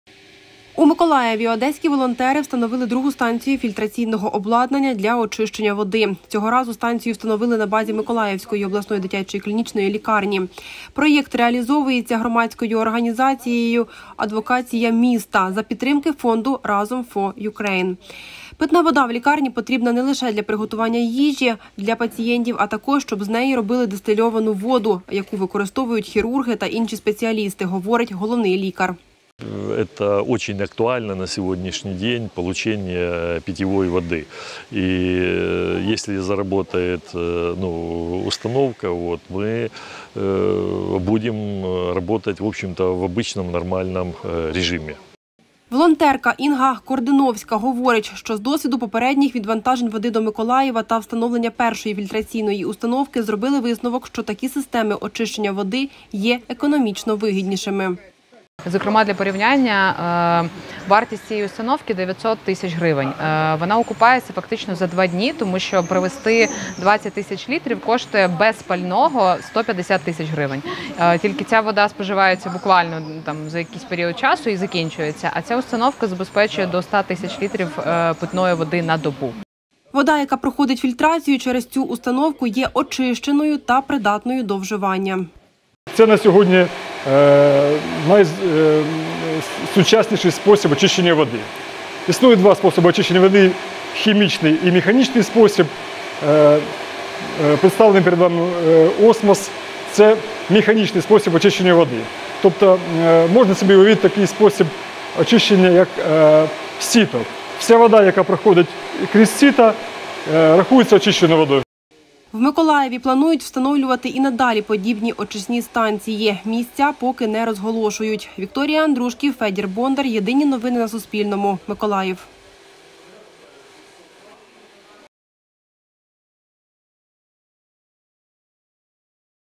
У Миколаївській лікарні запрацювала станція з очищення води
У Миколаєві волонтери з Одеси встановили другу станцію фільтраційного обладнання для очищення води. Встановили обладнання на базі обласної дитячої лікарні. Вона зможе давати 100 кубічних метрів чистої води за добу.

Автори - Вікторія Андрушків, Федір Бондар.